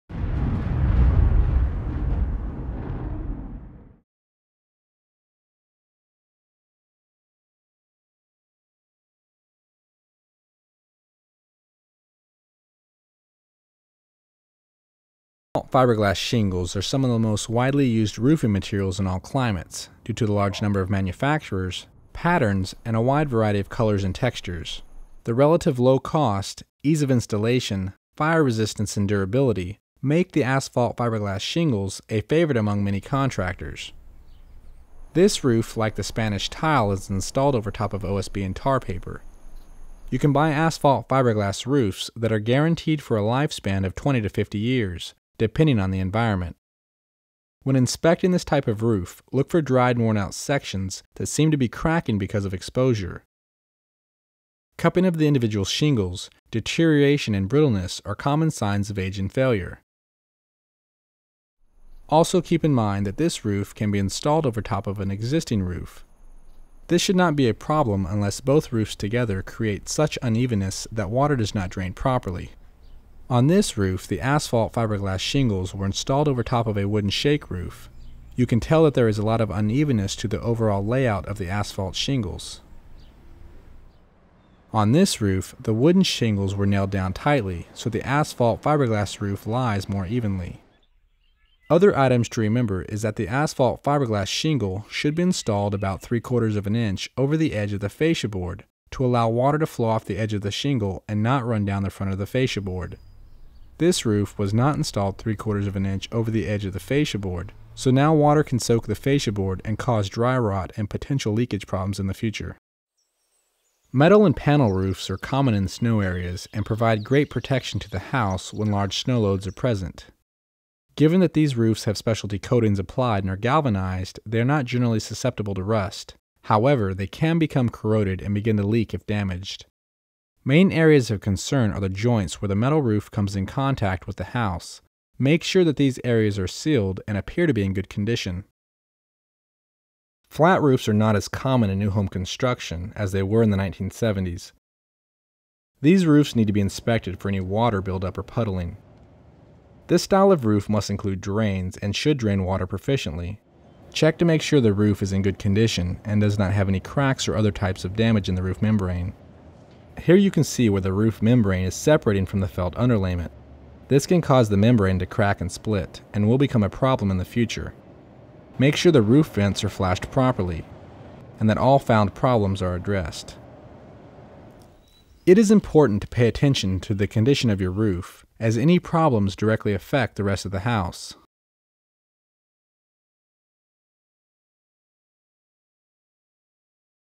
Home Inspection - Roofs - Part 3 of 3
When evaluating a new home, or your existing one, a key item of concern is the condition of the roof.  All roofs will eventually leak and will need to be repaired.  These repairs can cost several hundred dollars, with full replacements ranging anywhere from $5000 to well over $50,000.  Since the roof covers about 40% of the home's exterior, providing the majority of protection against the elements, semiannual roof inspections should be conducted.

Throughout warmer areas of the United States many residential roofs consist of cement or clay tiles, and natural stone such as slate.  Other materials include man-made composites that look like stone or clay, but weigh less, as well as a number of wooden shake and shingle configurations.  In addition, there are metal roofs, which are designed to look like shingle or tile roofs.

In colder regions, flat metal roofs are often used in order to deal with heavy snow loads.  These roofs come in long panels of sheet metal.  Most of these specialty metals have been coated to resist the elements so as to further their serviceability.  On flat roofs often there are long, continuous rolls of felt membrane and special asphalt fiberglass materials that have been bonded together to form a continuous water barrier.  Finally with the almost endless variation in color, style, and price, the asphalt fiberglass shingle is the favorite among most homeowners.